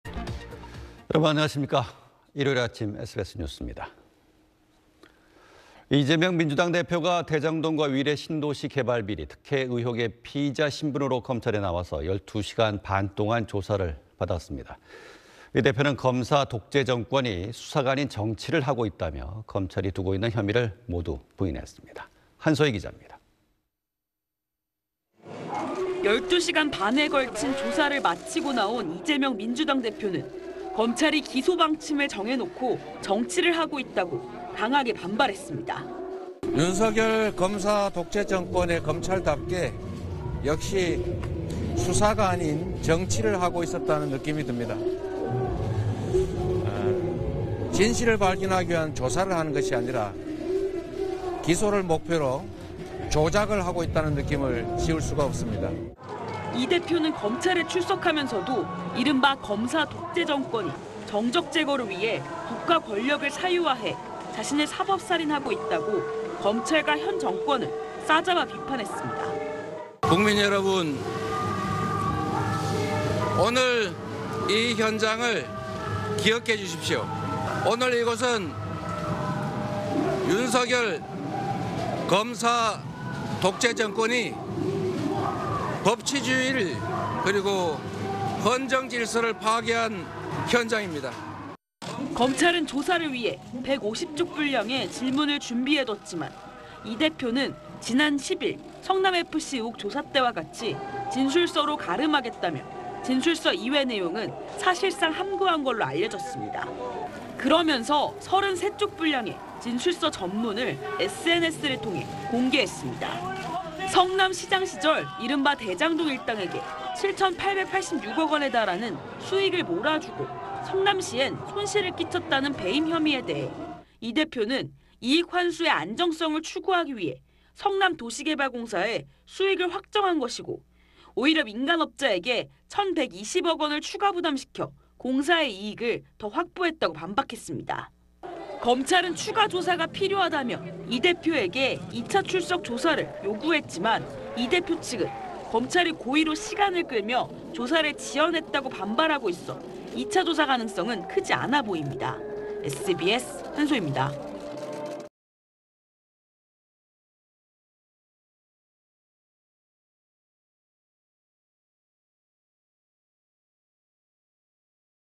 이재명 "검사가 정치"...검찰 "2차 출석 필요" / SBS
〈앵커〉

이재명 민주당 대표가 대장동과 위례신도시 개발비리, 특혜 의혹의 피의자 신분으로 검찰에 나와서 12시간 반 동안 조사를 받았습니다. 이 대표는 '검사 독재정권'이 수사가 아닌 정치를 하고 있다며, 검찰이 두고 있는 혐의를 모두 부인했습니다.

한소희 기자입니다.

〈기자〉

12시간 반에 걸친 조사를 마치고 나온 이재명 민주당 대표는 검찰이 기소 방침을 정해 놓고 정치를 하고 있다고 강하게 반발했습니다.

[이재명/민주당 대표 : 윤석열 검사 독재정권의 검찰답게 역시 수사가 아닌 정치를 하고 있었다는 느낌이 듭니다. 진실을 발견하기 위한 조사를 하는 것이 아니라 기소를 목표로 조작을 하고 있다는 느낌을 지울 수가 없습니다.]

이 대표는 검찰에 출석하면서도, 이른바 '검사 독재정권'이 정적 제거를 위해 국가권력을 사유화해 자신을 '사법 살인'하고 있다고 검찰과 현 정권을 싸잡아 비판했습니다.

[이재명/민주당 대표 : 국민 여러분, 오늘 이 현장을 기억해 주십시오. 오늘 이곳은 윤석열 검사 독재정권이 법치주의를 그리고 헌정질서를 파괴한 현장입니다.]

검찰은 조사를 위해 150쪽 분량의 질문을 준비해뒀지만, 이 대표는 지난 10일 성남FC 의혹 조사 때와 같이 진술서로 갈음하겠다며, 진술서 이외 내용은 사실상 함구한 걸로 알려졌습니다.

그러면서 33쪽 분량의 진술서 전문을 SNS를 통해 공개했습니다.

성남시장 시절 이른바 대장동 일당에게 7천886억 원에 달하는 수익을 몰아주고 성남시에는 손실을 끼쳤다는 배임 혐의에 대해, 이 대표는 이익 환수의 안정성을 추구하기 위해 성남 도시개발공사의 수익을 확정한 것이고, 오히려 민간업자에게 1천120억 원을 추가 부담시켜 공사의 이익을 더 확보했다고 반박했습니다.

검찰은 추가 조사가 필요하다며 이 대표에게 2차 출석 조사를 요구했지만, 이 대표 측은 검찰이 고의로 시간을 끌며 조사를 지연했다고 반발하고 있어 2차 조사 가능성은 크지 않아 보입니다.

(영상취재 : 전경배·김세경·김남성, 영상편집 : 이승진)